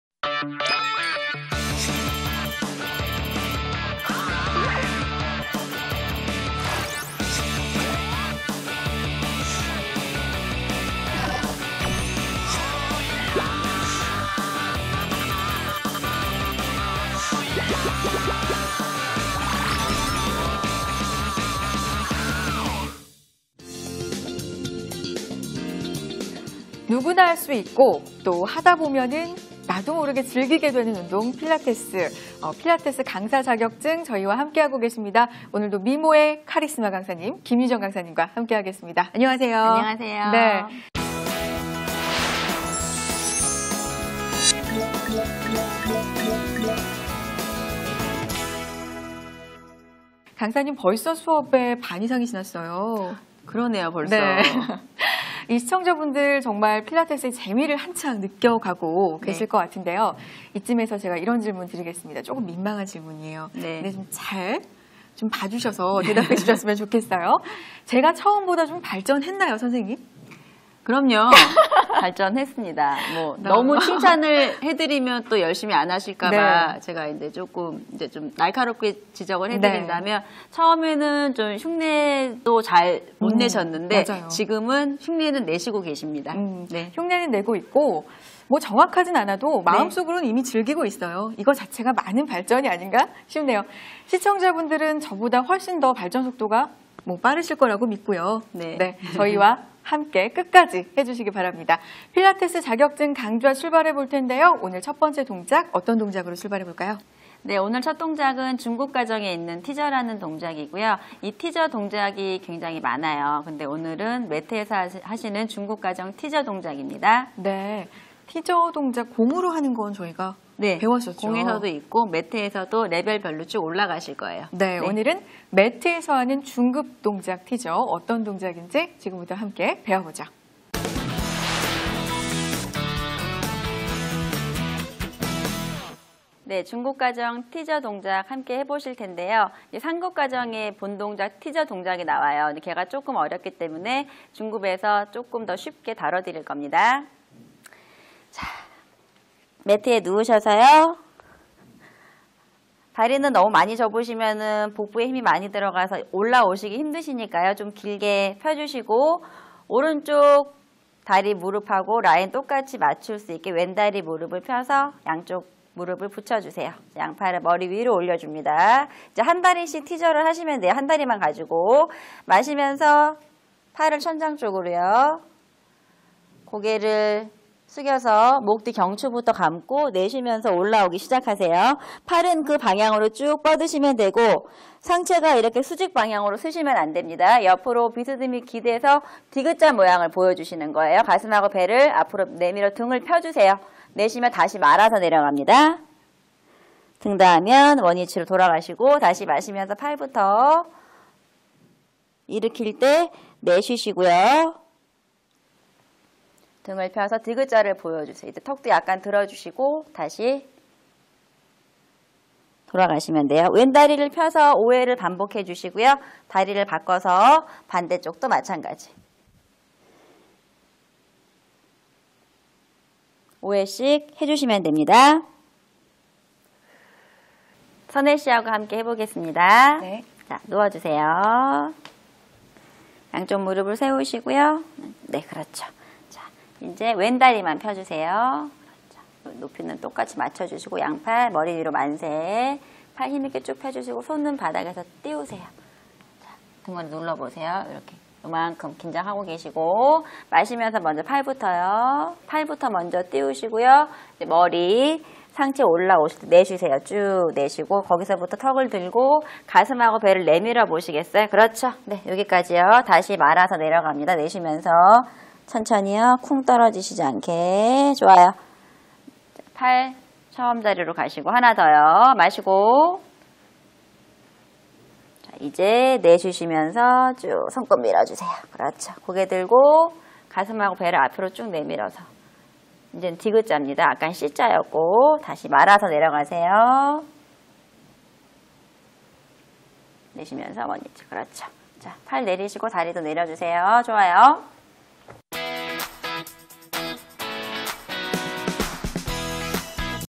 할수있다! 필라테스 10강(티져-중급, 롤 앤 터치, 업 앤 다운, 스파인 스트레치 포워드 등)
*제10강 티져-중급, 롤 앤 터치, 업 앤 다운, 스파인 스트레치 포워드 등
*강사 : 한국생활스포츠연합회 이사 김유정